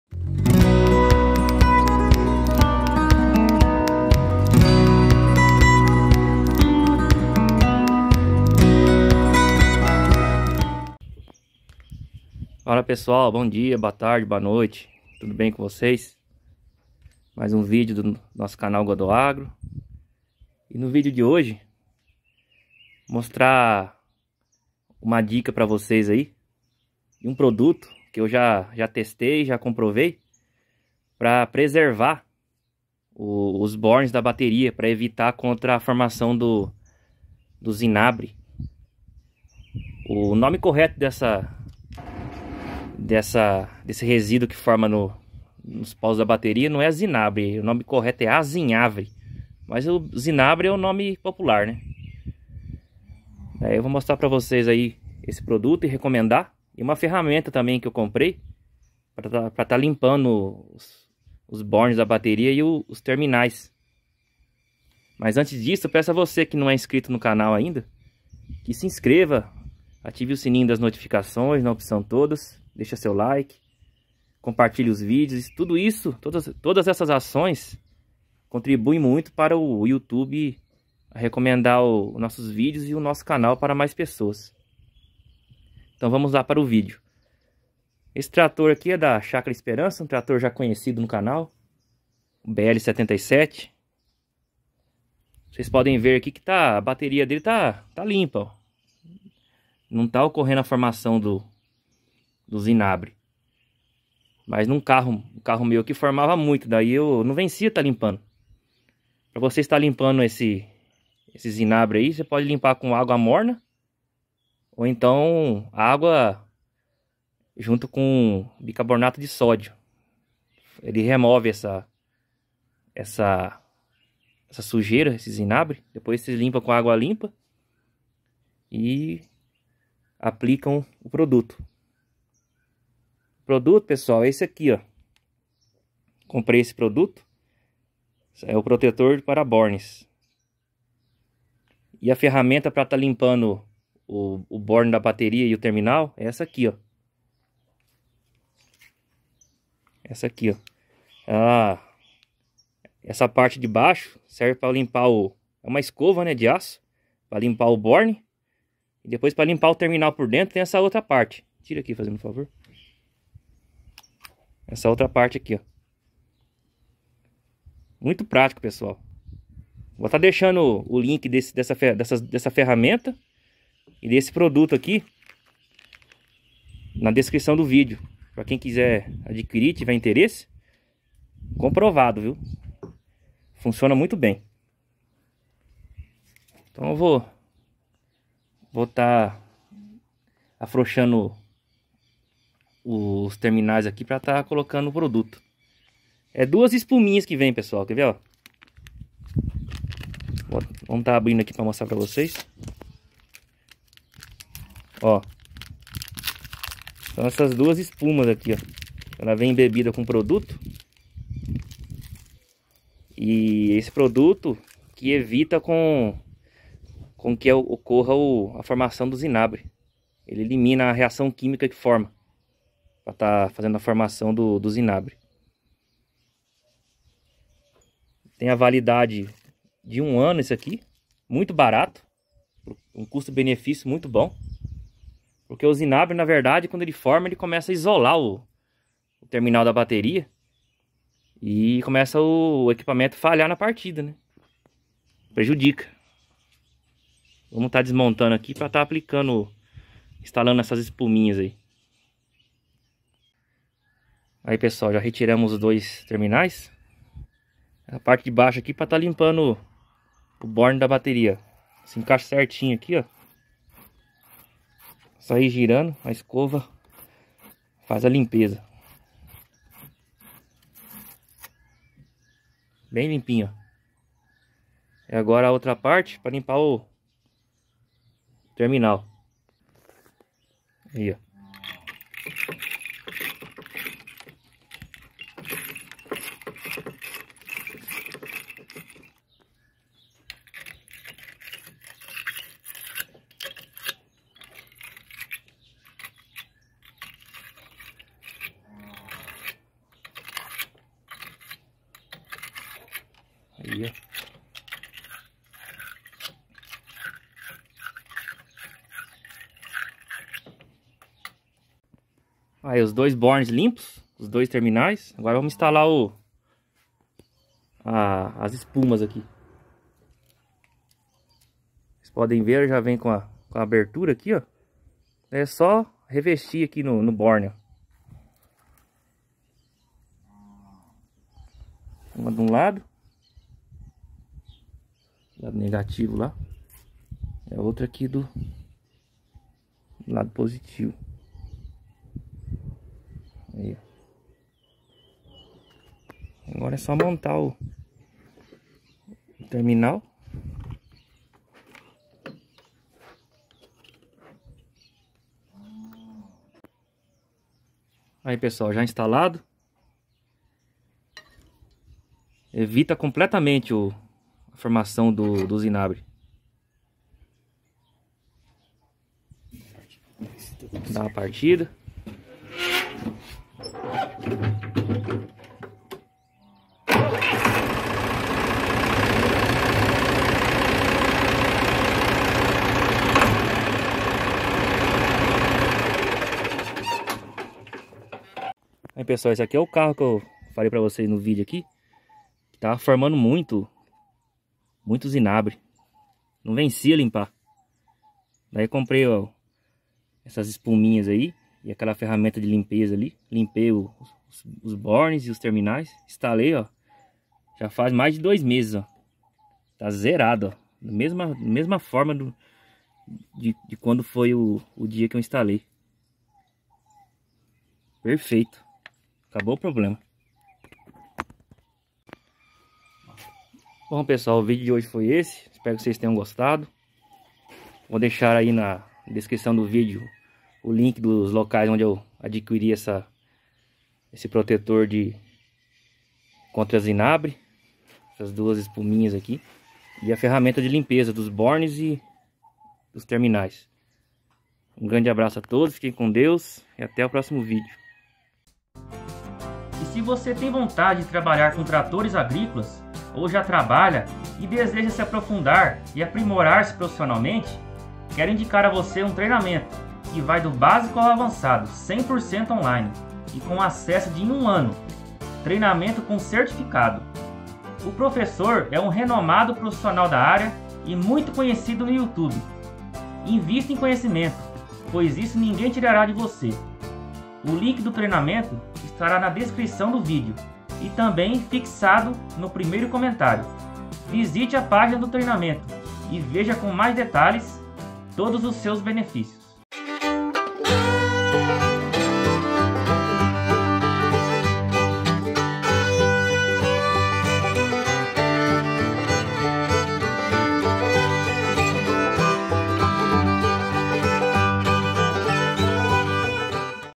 COMO PREVENIR O ZINABRE EM BATERIAS AUTOMOTIVAS?
Você já se deparou com aquela formação de resíduos esverdeada conhecida como zinabre nos terminais da bateria do seu carro ou trator? Essa formação pode causar mau contato, falhas elétricas, e até comprometer a vida útil da bateria. Neste vídeo, vou te ensinar como prevenir a formação do zinabre e também como fazer uma limpeza eficiente nos polos e terminais da bateria automotiva.

Vou te mostrar dois produtos: um deles previne o surgimento do zinabre e o outro faz a limpeza completa, deixando os polos e os terminais da bateria limpos. Ambos são fáceis de usar e super eficientes para manter a bateria do seu veículo funcionando perfeitamente.

🔧 Produtos mencionados no vídeo:

✅ Produto para Prevenção do Zinabre: Mantenha os terminais da sua bateria protegidos contra a corrosão.

✅ Quer se aperfeiçoar ou aprender do zero a ser um operador de máquinas agrícolas?

🔻🔻CLIQUE AQUI PARA CONHECER O CURSO🔻🔻